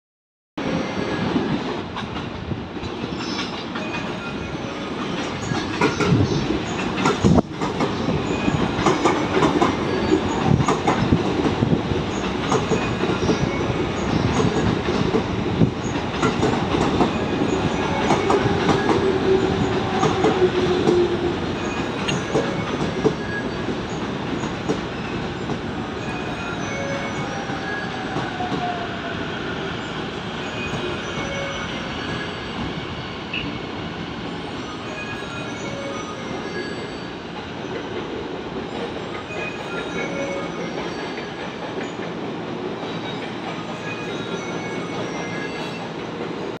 E231系0番台マト117編成+マト134編成上野駅入線・到着
1788H
常磐線快速上野行

折り返し上野始発
1889H
常磐線快速取手行